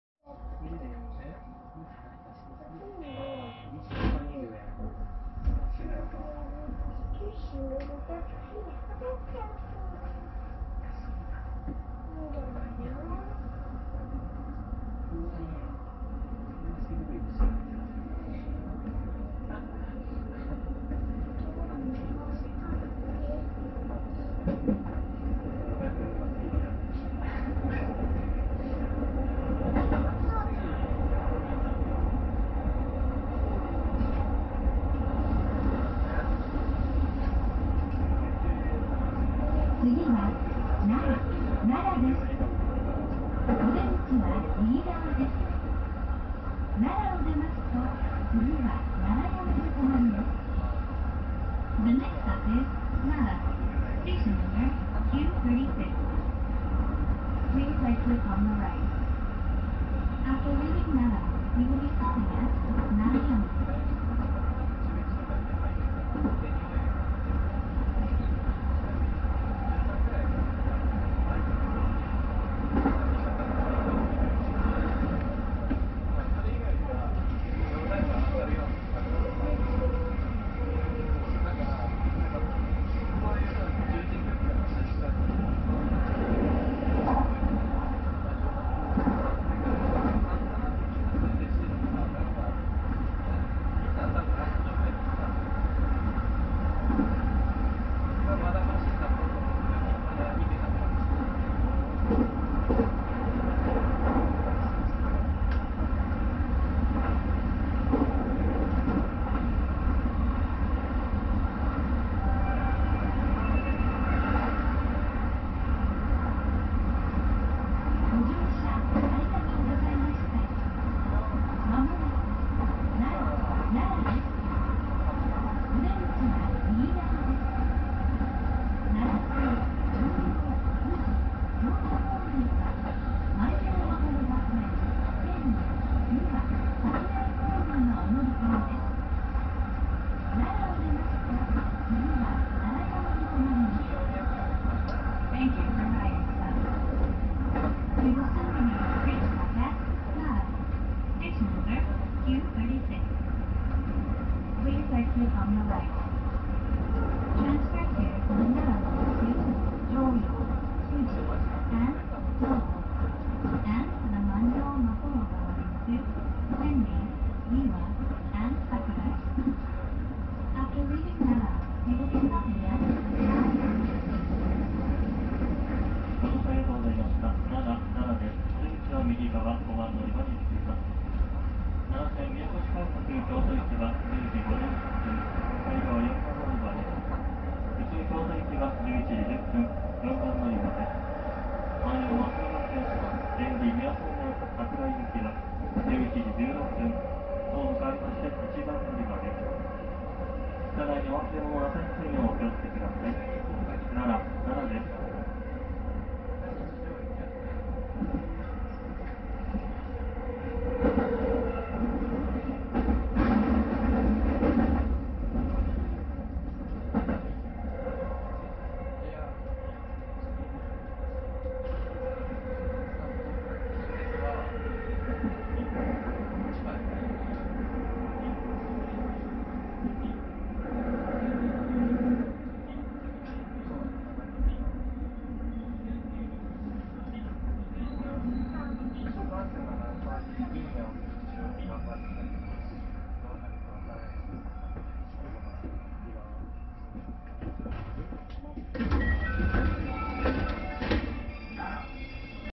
郡山→奈良 21.11.06 大和路線(大和路快速) JR西日本221系 八条新駅建設、高架化工事 4k前面展望 撮影・録音：Zenfone7 Pro(HDR)
■0:00 郡山(Koriyama,JR-Q34)
 |-1:51 (新駅予定・高架化工事)(,JR-Q35)
■3:28 奈良(Nara,JR-Q36,D21)

八条・大安寺周辺地区まちづくり基本構想(JR八条新駅(仮称・奈良-郡山間)設置工事)